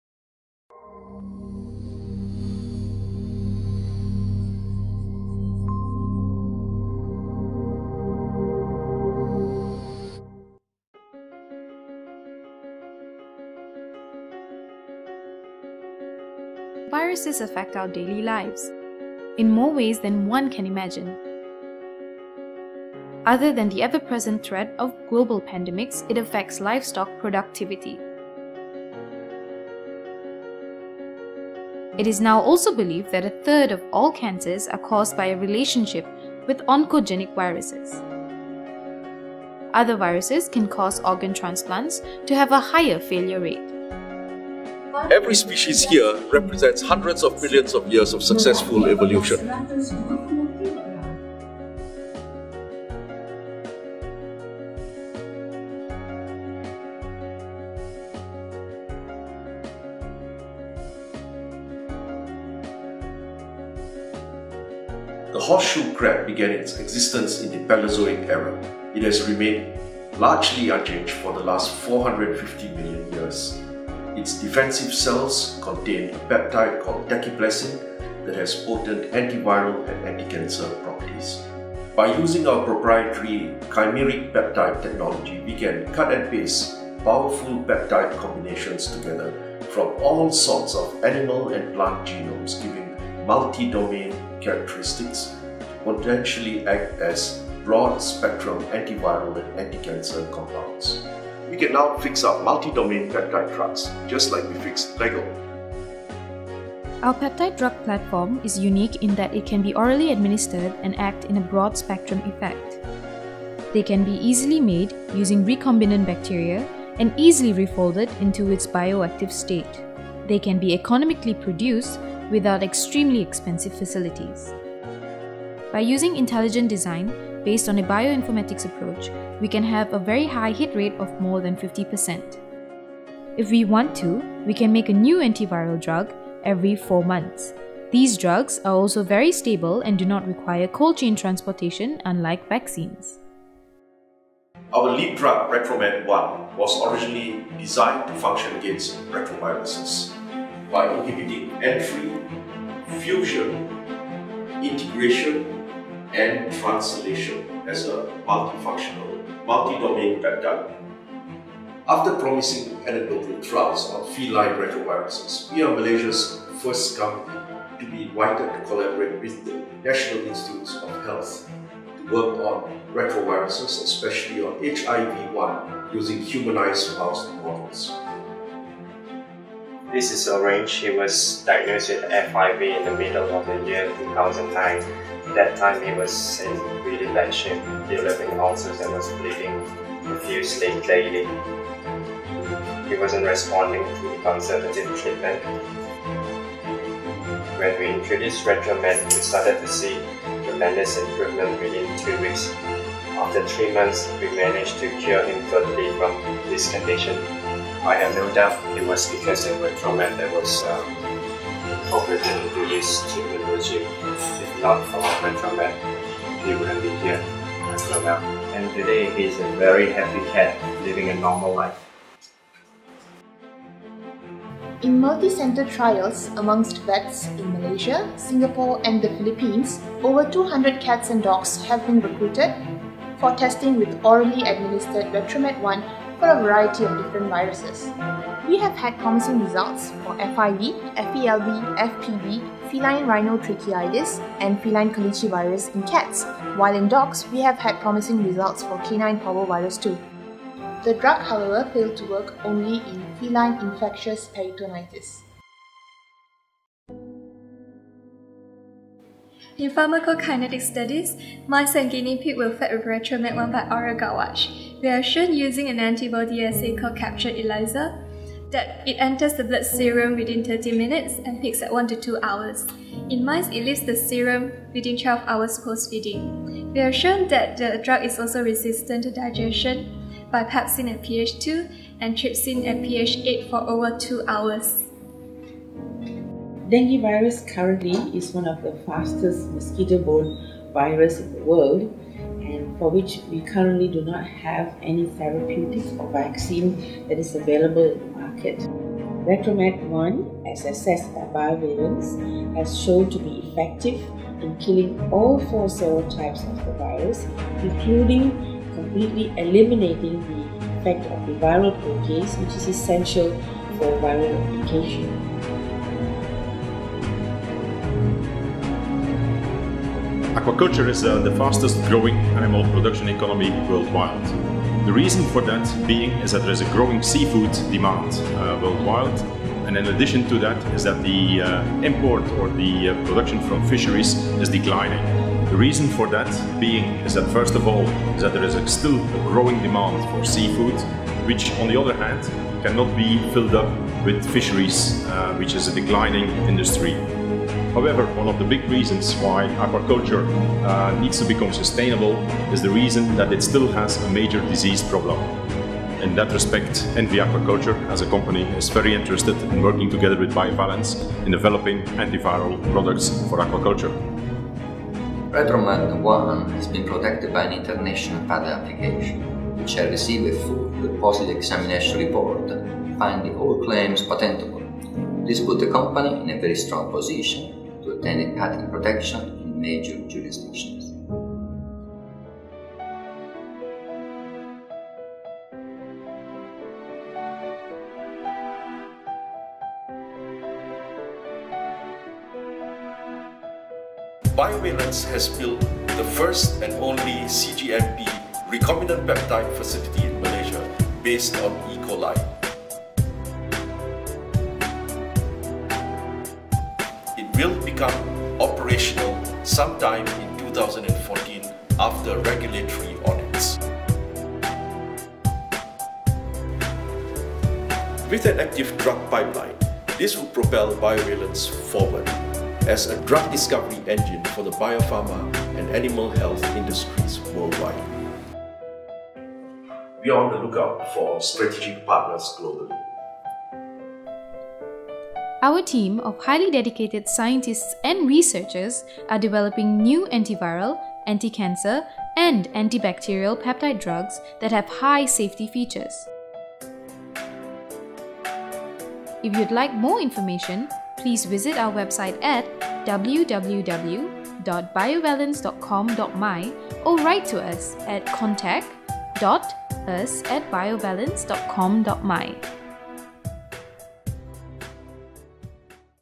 BioValence 2014
BioValence Sdn.Bhd. based in Kuala Lumpur is developing  cutting-edge oral-delivery broad-spectrum antiviral and anticancer biologics.

The problem is that peptides are usually too small and too easily digested by digestive enzymes. We can 'cut and paste' genes of interest using a LEGO-like platform technology to produce multi-domain fusion proteins capable of being orally administered. Using powerful bioinformatics search engines, we select compelling candidate genes with the result of a higher than 50% 'hit rate'. We now possess an almost unlimited capability of producing new antiviral and anticancer candidates using our novel proprietary platform as a drug discovery engine that will be soon supported by an in house- GMP manufacturing facility for clinical batches.

RetroMAD1 is a 41.2 kDa Chimeric Anti Microbial (ChAMP) .By using Super Critical  Fluid Drying (SCFD) RetroMAD1 can also be  turned into a free flowing micronized powder form in addition to its normal liquid format.

In our trials for RetroMAD1 as a broad spectrum antiviral, we have indeed shown that RetroMAD1 exhibits antiviral properties in viral infections in various species. From shrimp viruses  in aqua-culture (completely stopping mortality); to improving survival and quality of life of our companion animals from Feline Immunodeficiency (FIV) Feline Leukemia Virus (FeLV) and Canine Parvovirus 2, (with symptomatic recovery rates as high as 70 -- 80%); to in vitro testing against human viruses such as Herpes Simplex Virus- 1&2 and all 4 Dengue Virus serotypes (showing viral reduction to as high as 100% in HSV2 and DENV2&4). Our drug has shown promising potential and opens the door for many more future treatment possibilities including Chikungunya. HIV-1 humanized mouse trials are with the National Institutes of Health (USA) will be done in 2014.